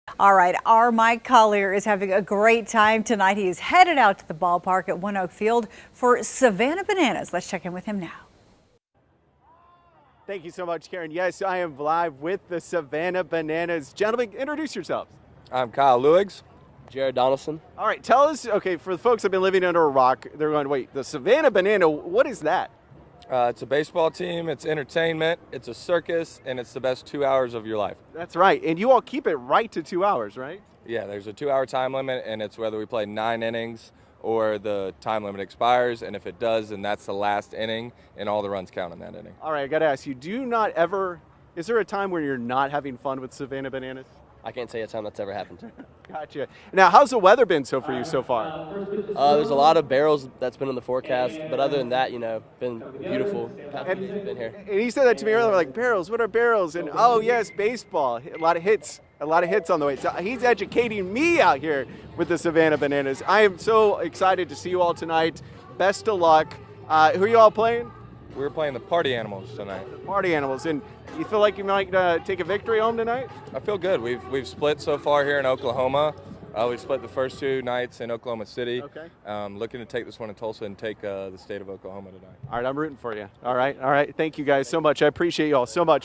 Savannah Bananas preparing for first Tulsa game
The exhibition team gained national attention due to its clever name, creative and unusual baseball games and trendy social media videos. With 6.8 million TikTok followers, the team announced a World Tour bringing Banana Ball to 33 cities and 20 states. The Bananas are officially in Tulsa for the first time.

Chief Meteorologist Mike Collier reports.